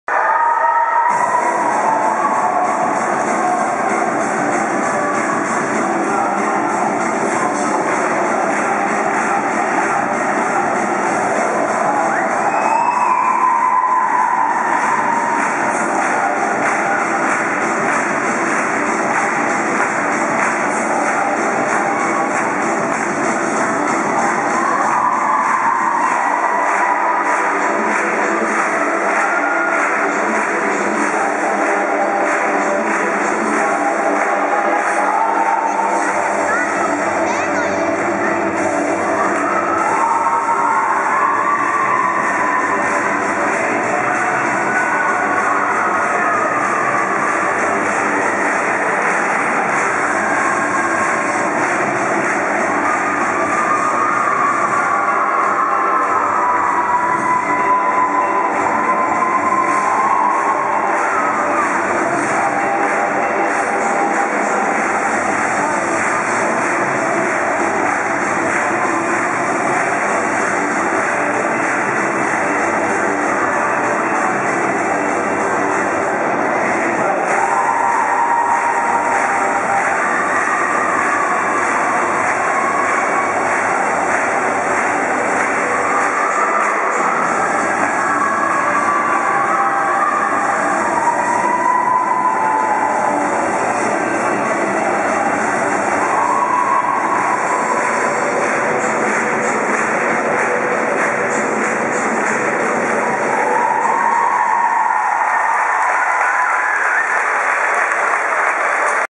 Državno 2017 LUCIJA & MARKO
LUCIJA ROGINIĆ & MARKO ŠOBAK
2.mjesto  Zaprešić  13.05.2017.
26.Prvenstvo Republike Hrvatske u akrobatskom rock'n'rollu
Akrobatski rock'n'roll sport 21.stoljeća